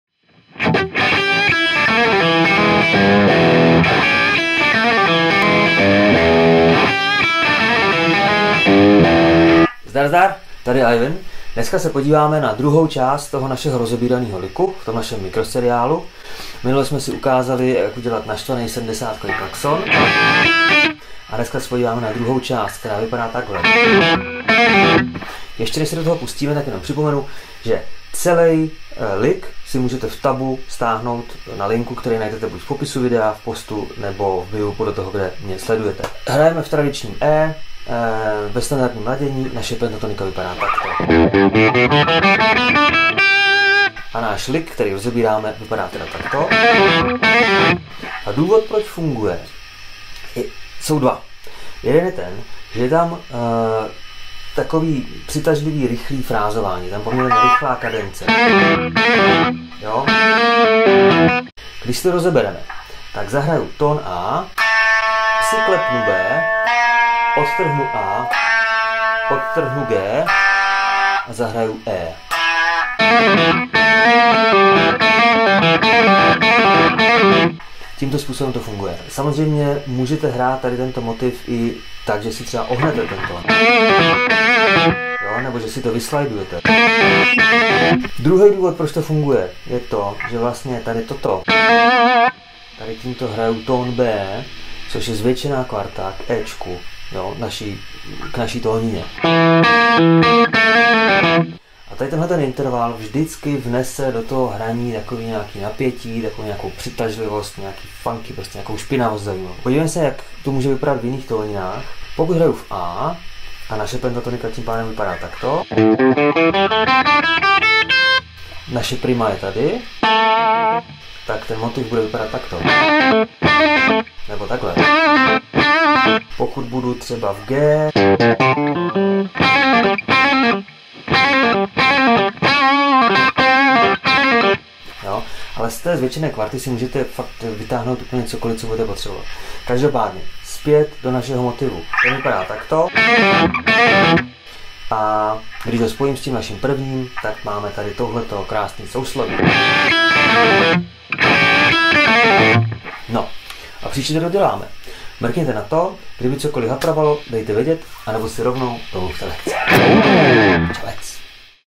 KytaraPodleAivna: Angry Blues Lick | Lekce 2
Fact: bez zvětšené kvarty to nejede. Schválně mrkněte, jak rozhýbe náš lick z minulého týdne. Druhý díl KytaraPodleAivna je tady!